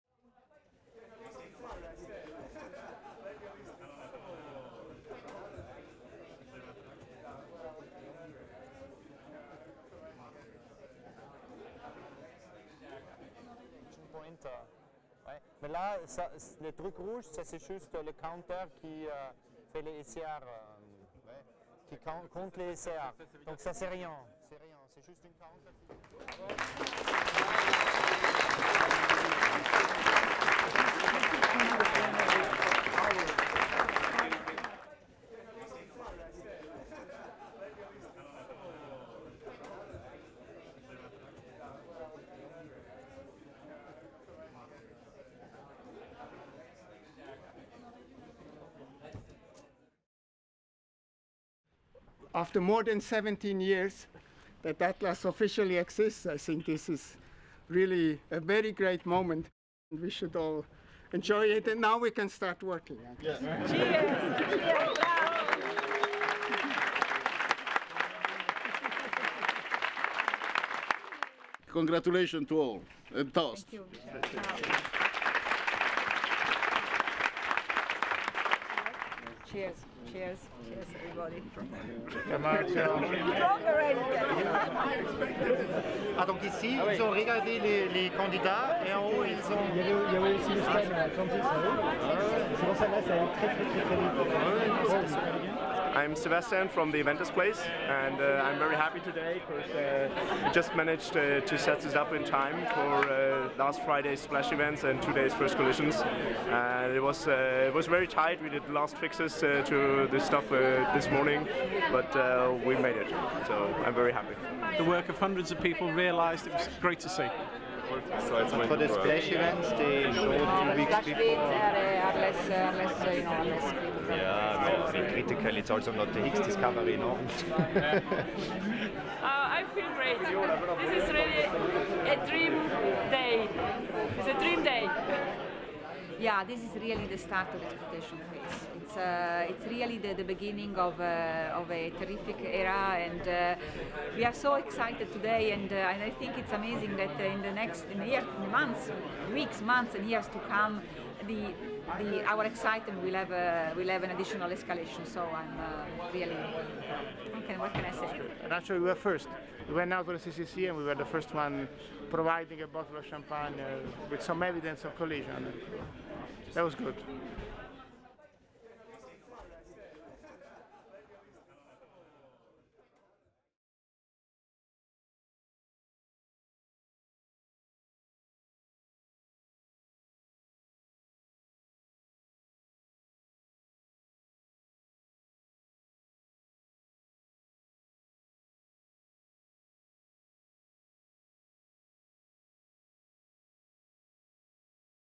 First ATLAS Collisons, 23rd November 2009
Footage of the control room as ATLAS observed the first proton-proton collision event candidate on 23 November 2009 at 14:22